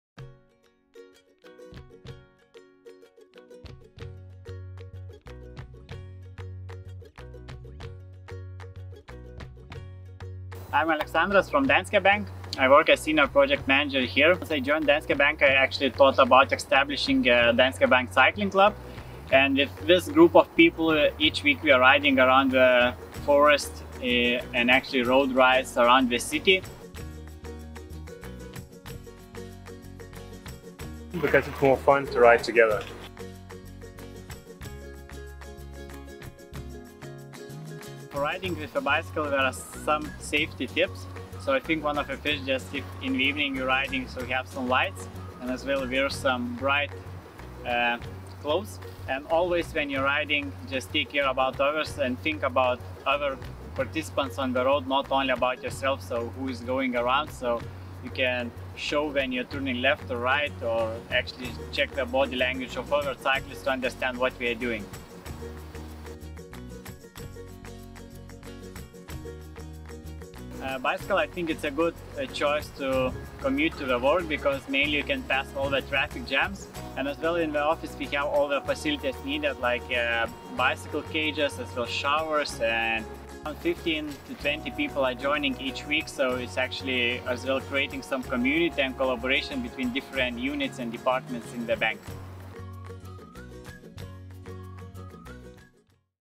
Danske Spotlight: meet our Cycling club
What does Danske Bank's culture consist of? The latest game consoles, table tennis, graffiti office walls? Nope! The foundation of our culture is a vibrant community in which everyone can be themselves, create and prosper. We know them very well and we'd like to introduce our communities to you as well. 👋